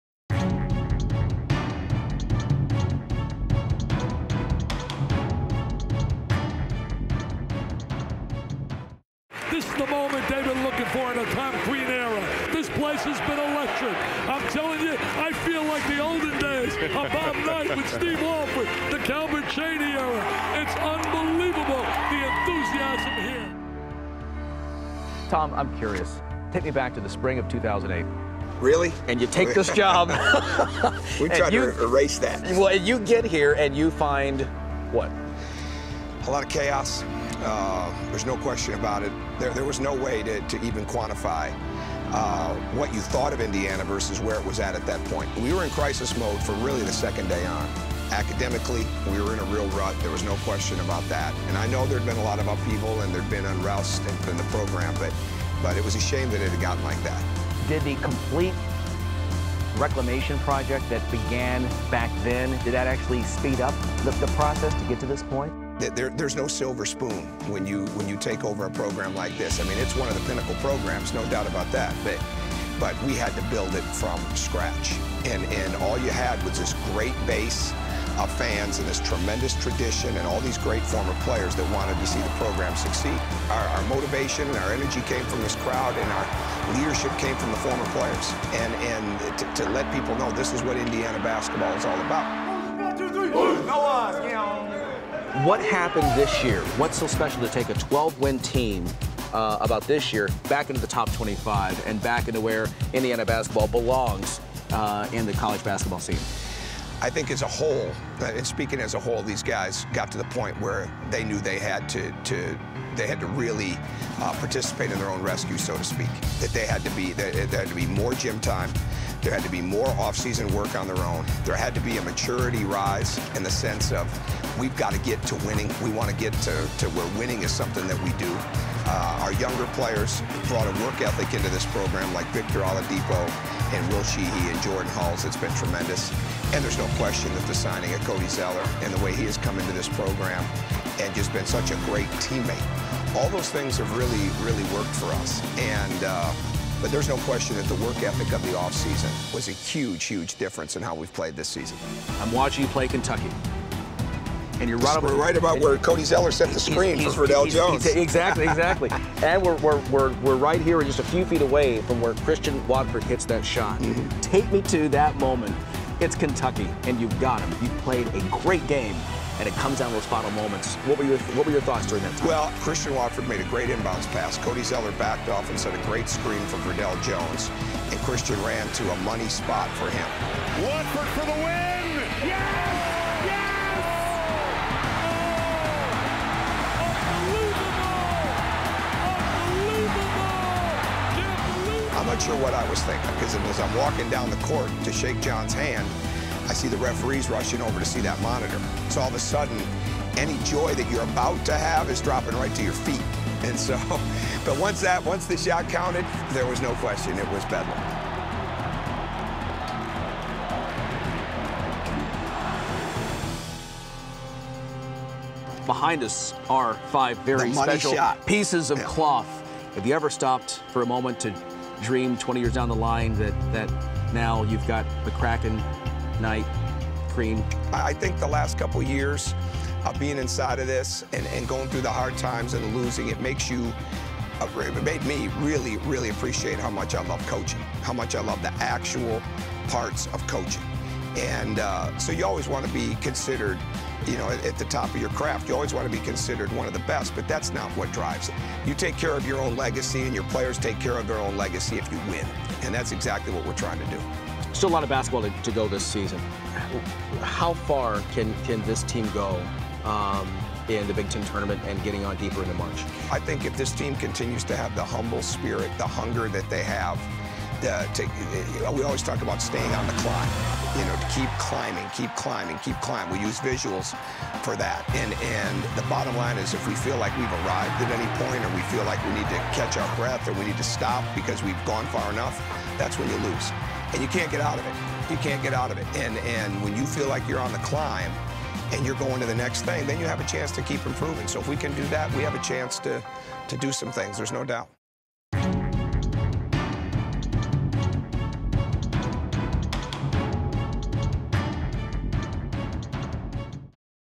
Tom Crean Feature
Indiana University's head coach Tom Crean shares his insight on the direction of the college basketball program.  This piece was edited by Peter Denis for NCAA.com.